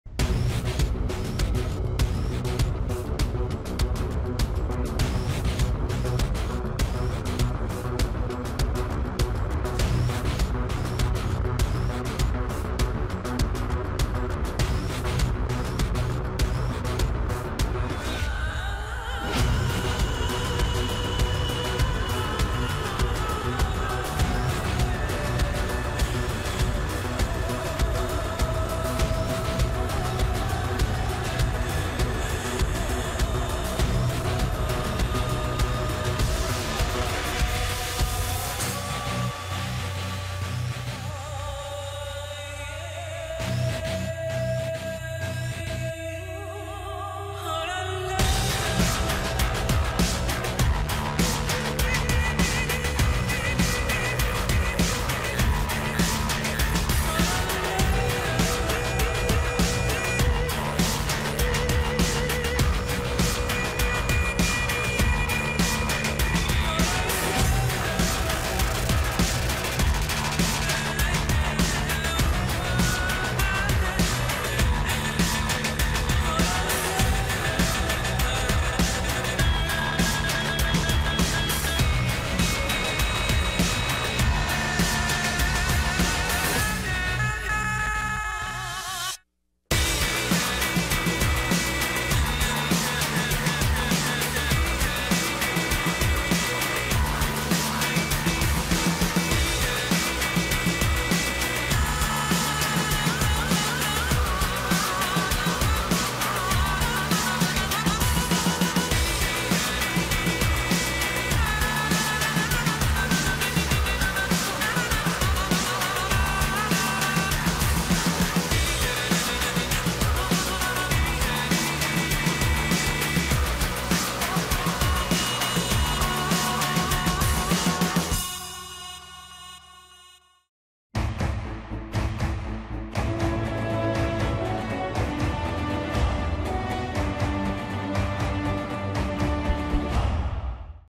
LaLiga Skills Jornada 26: de Frutos, Pedri & Joaquín
Controles, taconazos, regates, caños... ¡No te pierdas los mejores skills de la Jornada 26 de LaLiga Santander 2021/2022